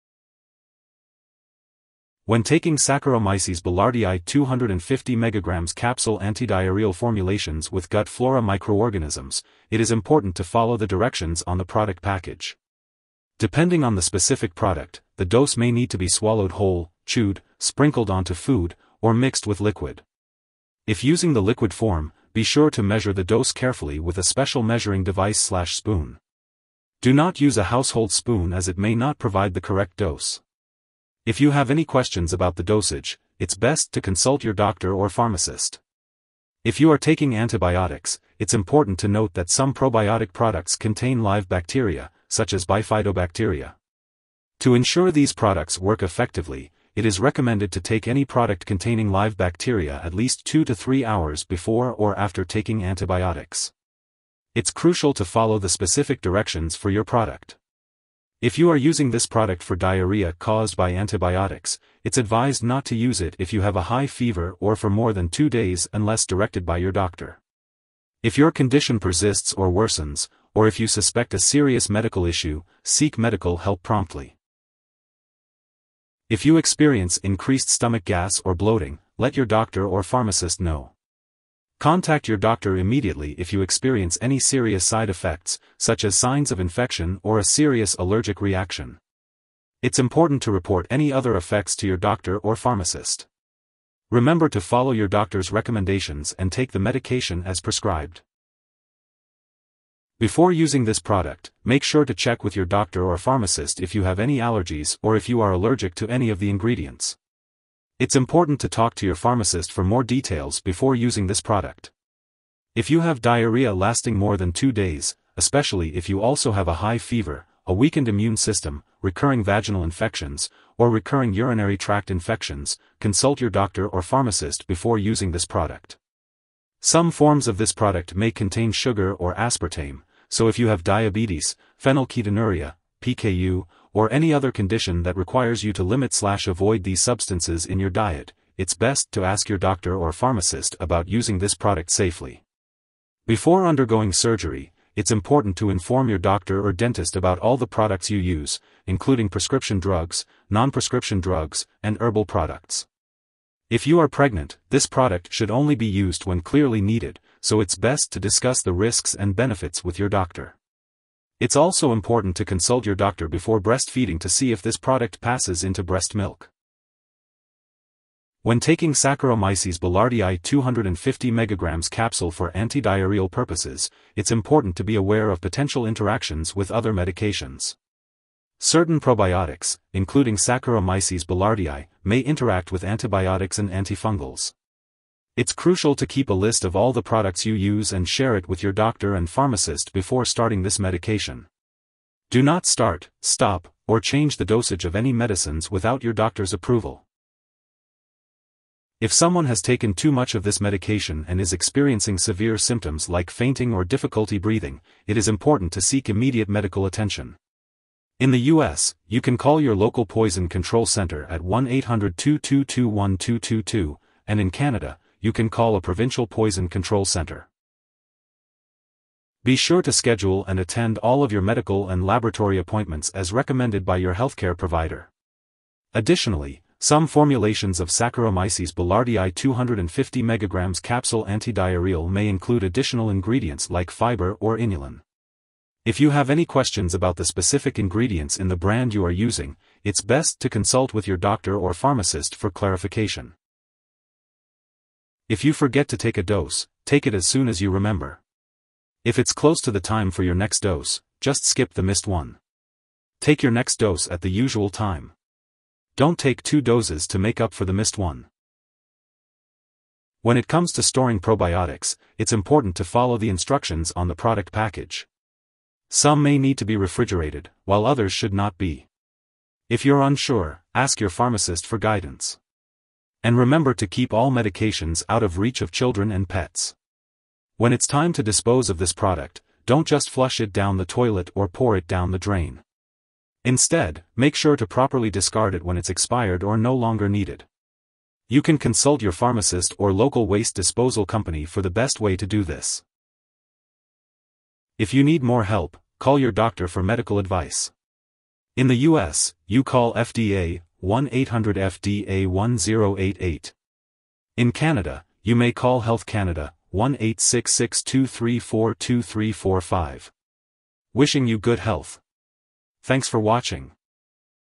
How to use Saccharomyces Boulardii 250 Mg Capsule Antidiarrheal Formulations With Gut Flora Microor
When taking Saccharomyces Boulardii 250 Mg Capsule Antidiarrheal Formulations With Gut Flora Microorganisms, it is important to follow the directions on the product package. Depending on the specific product, the dose may need to be swallowed whole, chewed, sprinkled onto food, or mixed with liquid. If using the liquid form, be sure to measure the dose carefully with a special measuring device/spoon. Do not use a household spoon as it may not provide the correct dose. If you have any questions about the dosage, it's best to consult your doctor or pharmacist.

If you are taking antibiotics, it's important to note that some probiotic products contain live bacteria, such as bifidobacteria. To ensure these products work effectively, it is recommended to take any product containing live bacteria at least 2 to 3 hours before or after taking antibiotics. It's crucial to follow the specific directions for your product.

If you are using this product for diarrhea caused by antibiotics, it's advised not to use it if you have a high fever or for more than 2 days unless directed by your doctor. If your condition persists or worsens, or if you suspect a serious medical issue, seek medical help promptly.
e784